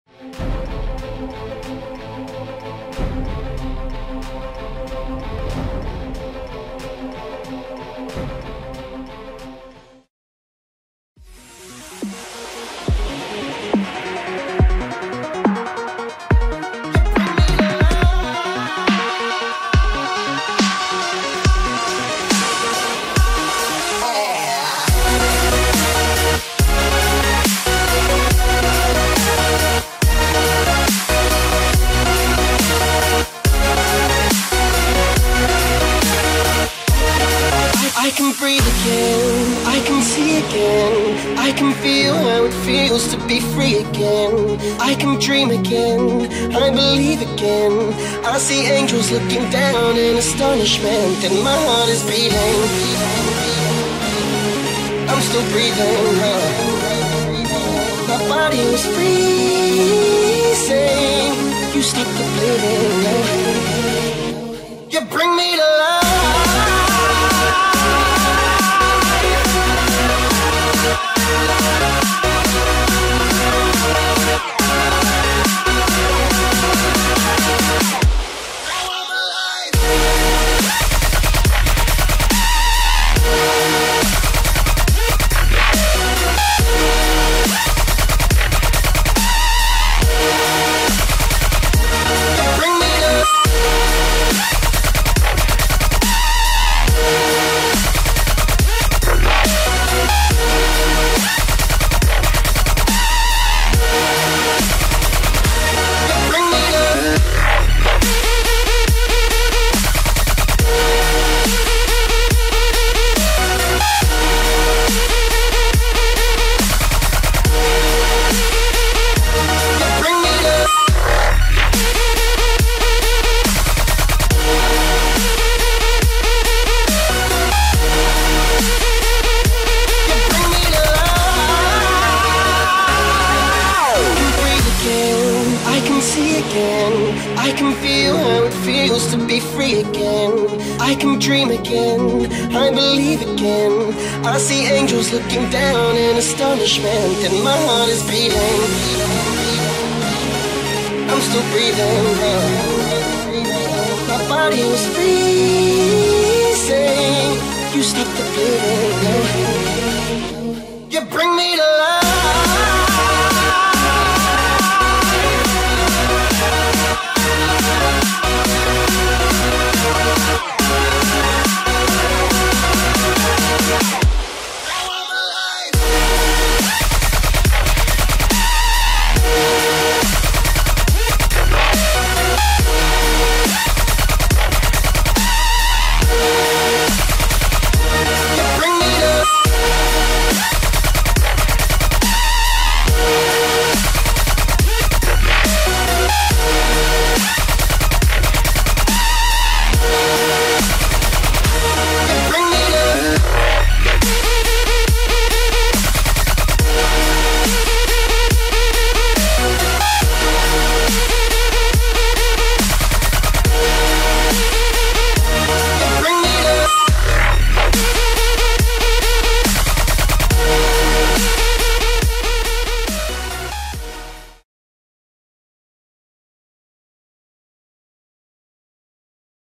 Gimp 2 Speed Art - Cyborg
Clip Speed - 1200 %
Hello :) welcome in my cyborg speed art if u have any question message me .
Hope this was entertaining to watch.

I used dubstep music because its good to cyborg :)